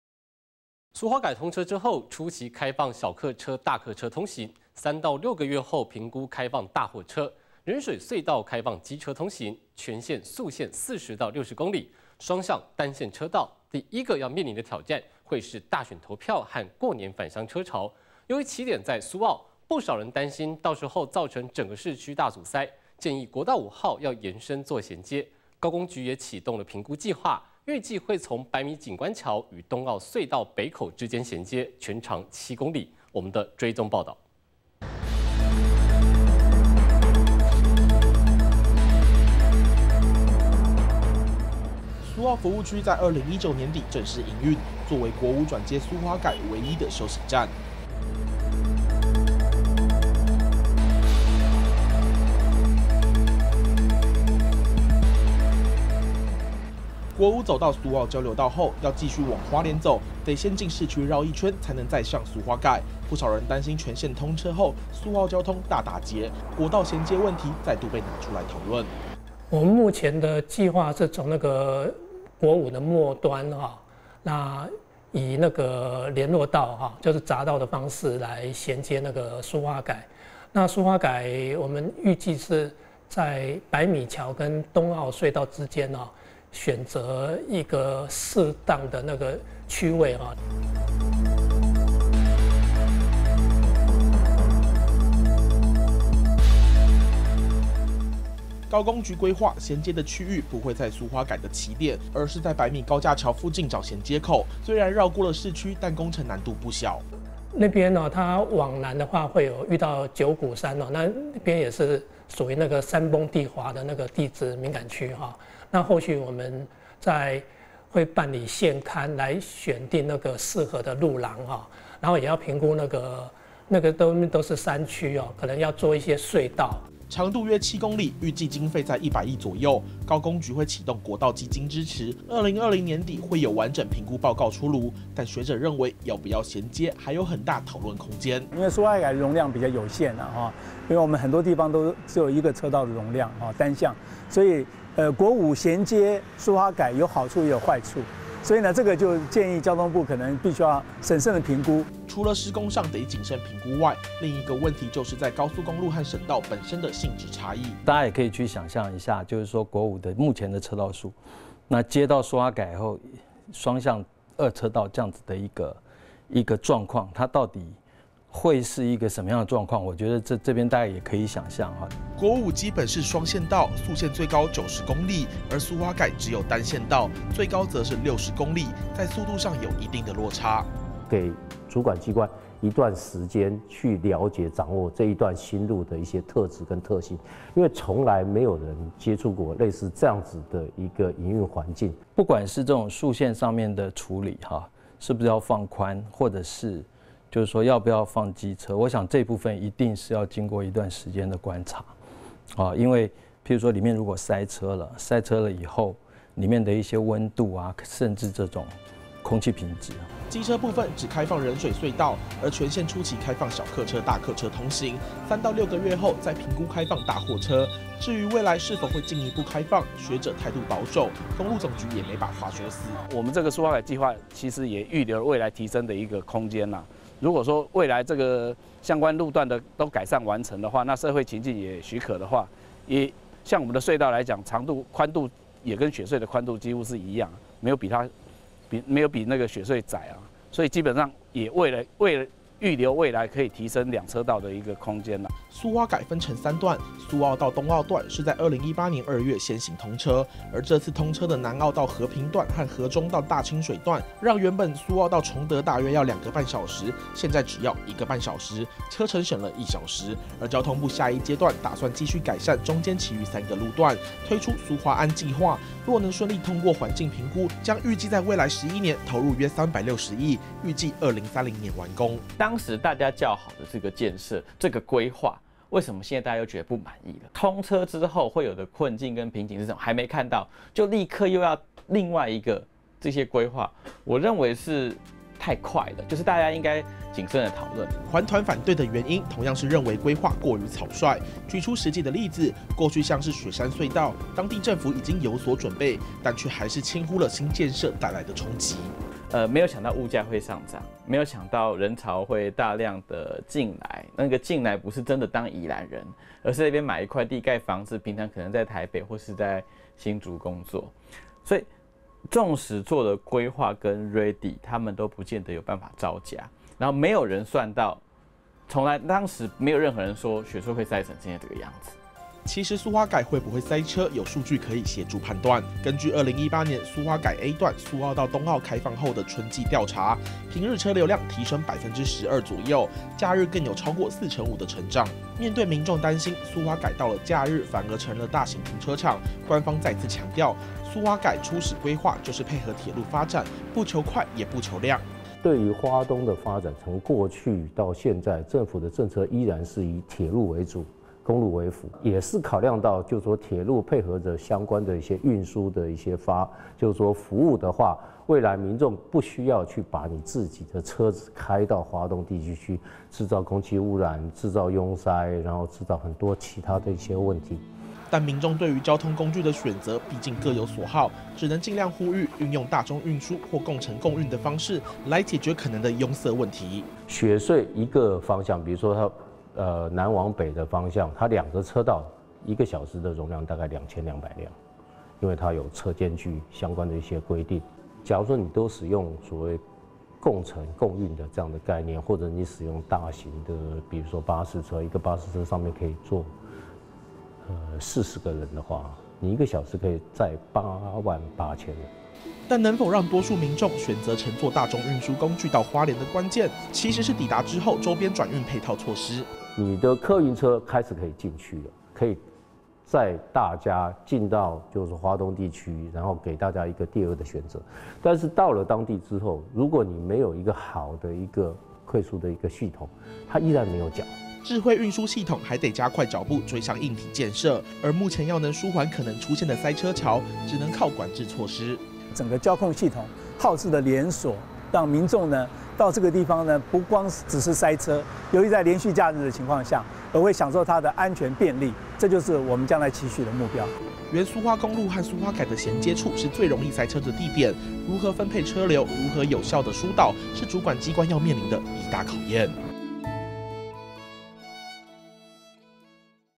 熱線追蹤-國五銜接 評估啟動
蘇花改通車後，初期開放小客車，大客車通行，3到6個月後評估開放大貨車，仁水隧道開放機車通行，全線速限40到60公里，雙向單線車道，第一個要面臨的挑戰，會是大選投票和過年返鄉車潮，由於起點在蘇澳，不少人擔心，到時候造成整個市區大阻塞，建議國道5號，要延伸做銜接，高公局也啟動評估計畫，預計會從白米景觀橋與東澳隧道北口之間銜接，全長7公里。